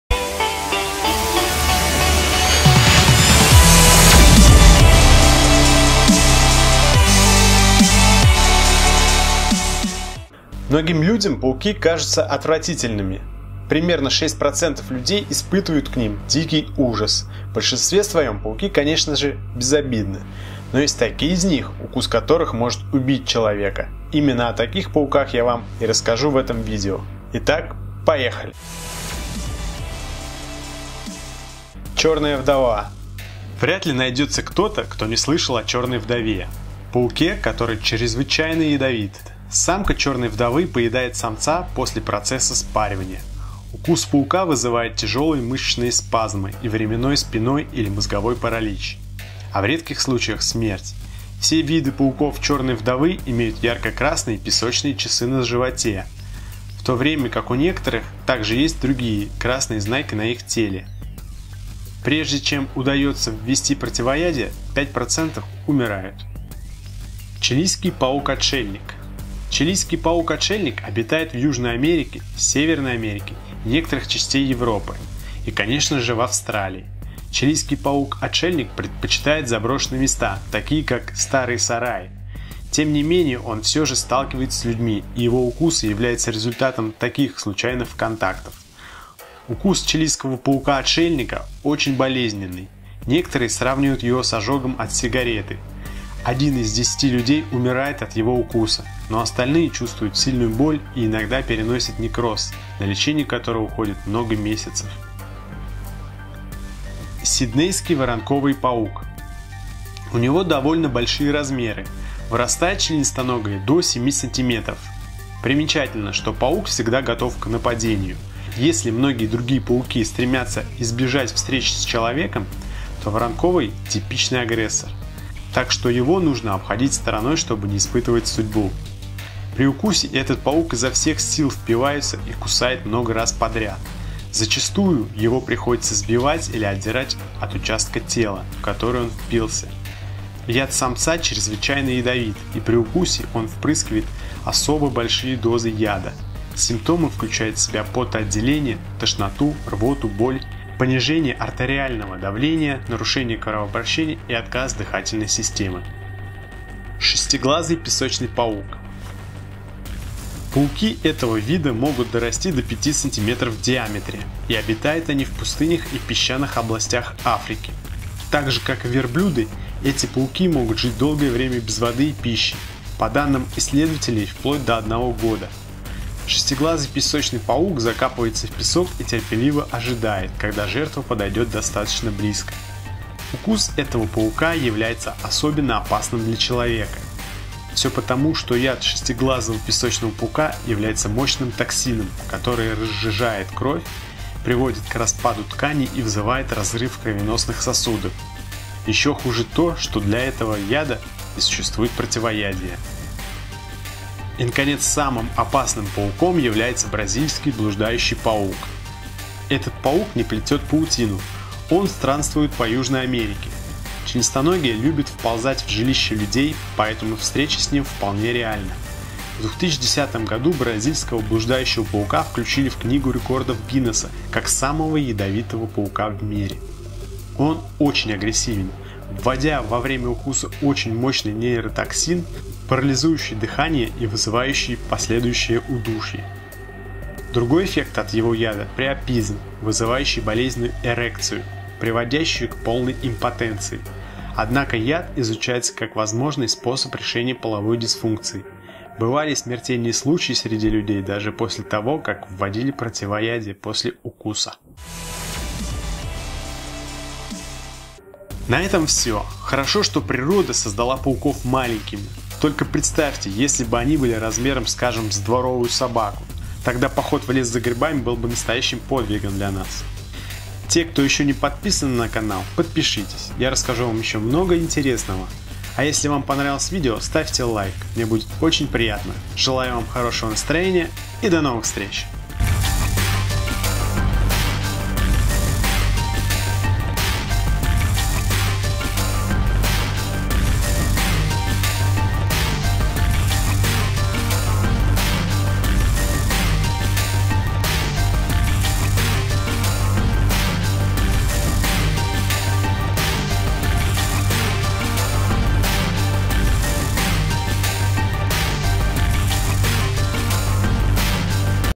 Самые опасные и ядовитые пауки в мире
Многим людям пауки кажутся отвратительными, примерно 6% людей испытывают к ним дикий ужас. В большинстве своем пауки конечно безобидны, но есть такие из них, укус которых может убить человека. Именно о таких пауках я вам и расскажу в этом видео.